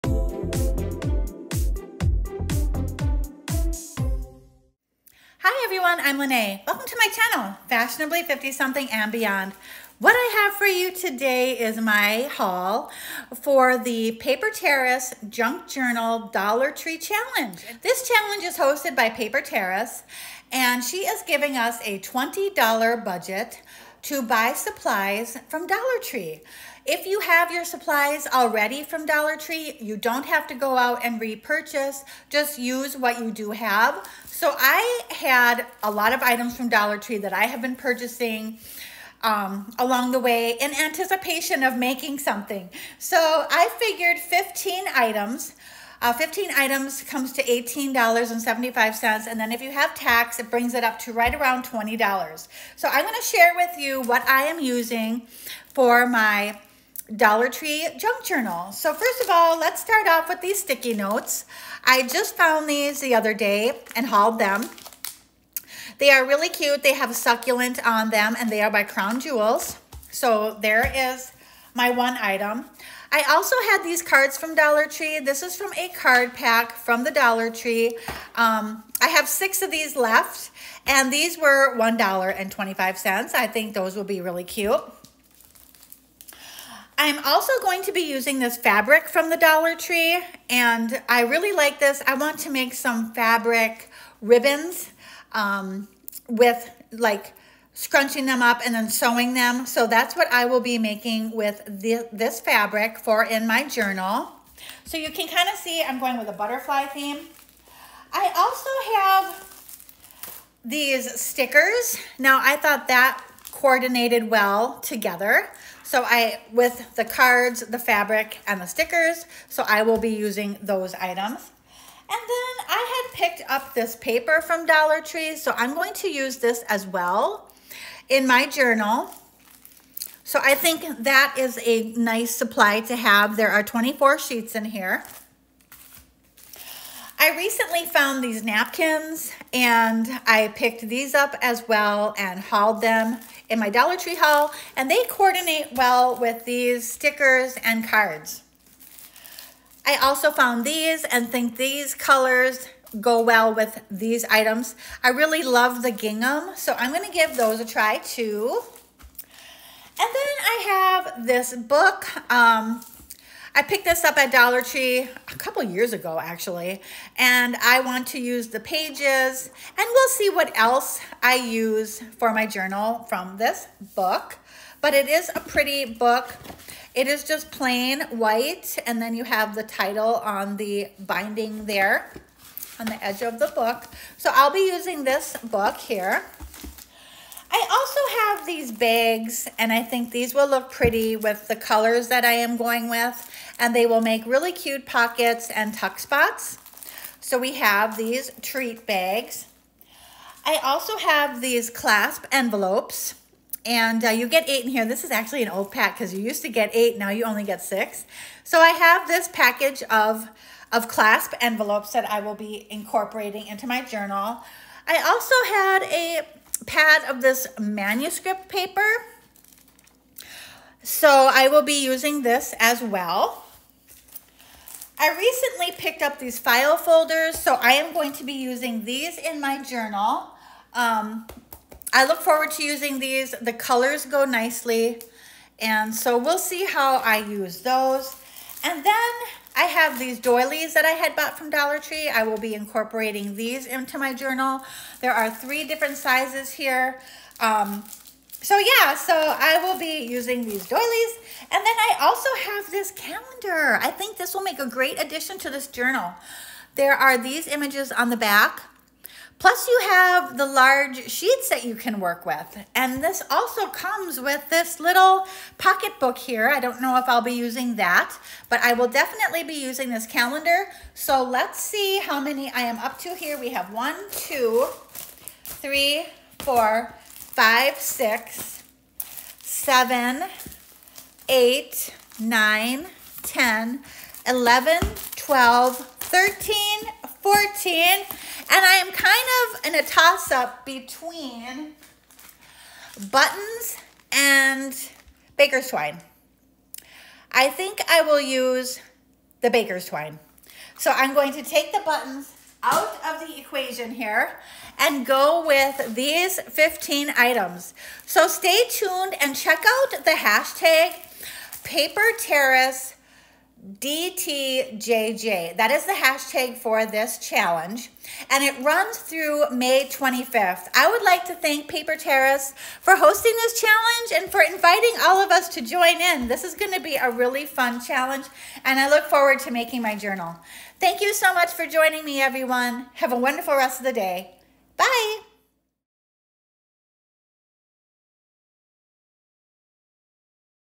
My Dollar Tree Supplies for paperterracedtjj March 1 2024
Hi everyone!  This is an open challenge hosted by @PaperTerrace!  She is allowing a $20 budget to buy your items from Dollar Tree to create a junk journal!  This challenge started on March 1, 2024, and runs until March 25, 2024.  Please come along and see what I will be using for my journal, and then search the hashtag to see what everyone else will be making!  Thank you @PaperTerrace for hosting this wonderful challenge!  Thank you for stopping by!  Have a wonderful day!